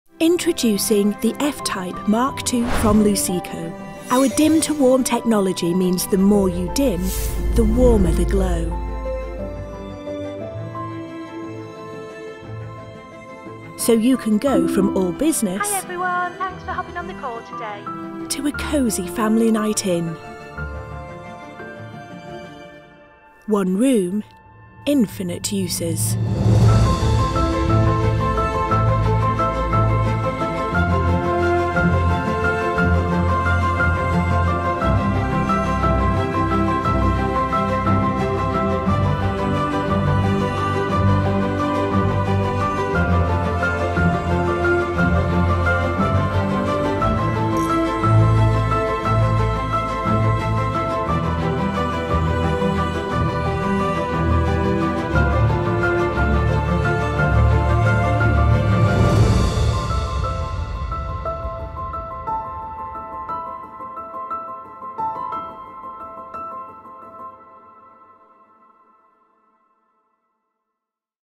💡Transform Your Space With Dim2Warm One Room, Infinite Possibilities
Welcome to an illuminating journey with @lucecogroup  as we explore the transformative power of Dim2Warm Lighting with The FType Mk2 Fire Rated LED Downlights.

Dim2Warm technology offers the perfect lighting solution for multi-use rooms, seamlessly transitioning from bright productivity to cozy relaxation with just a turn of a dimmer switch.

Transform your space with our new FType Mk2 Dim2Warm Downlights by Luceco Lighting and bring infinite possibilities to your rooms.

👍 Help our videos travel further!